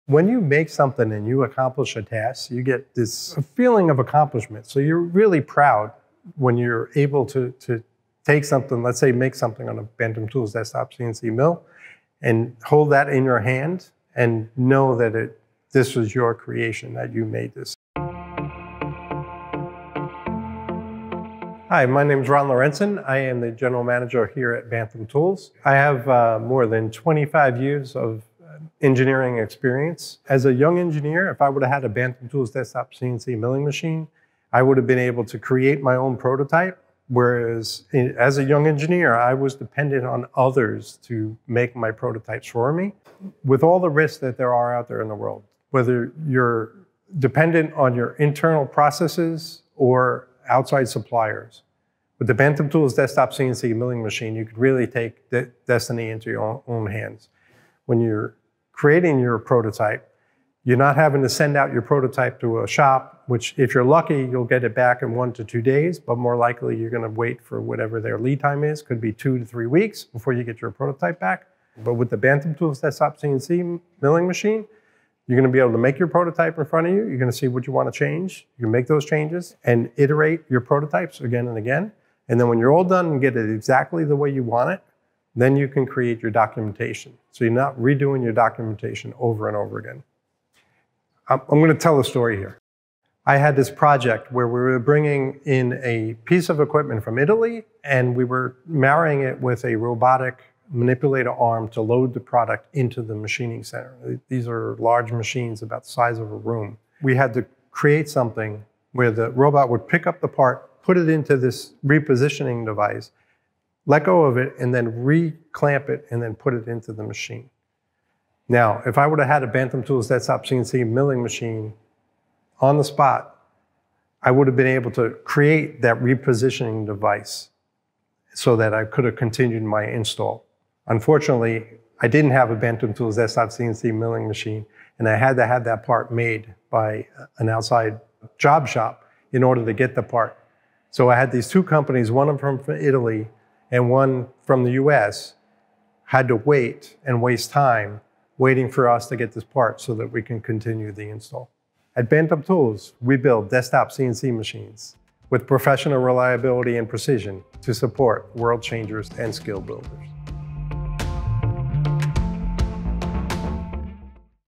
Bantam Tools: An Engineer's Perspective
Meet our General Manager, Ron Lorentzen. Ron brings over 25 years of engineering experience to the Bantam Tools team. We sat down with him to hear about why he believes the Bantam Tools Desktop CNC Milling Machine is critical to the prototyping process and how it empowers world changers and skill builders to be independent, resilient, and solve problems quickly.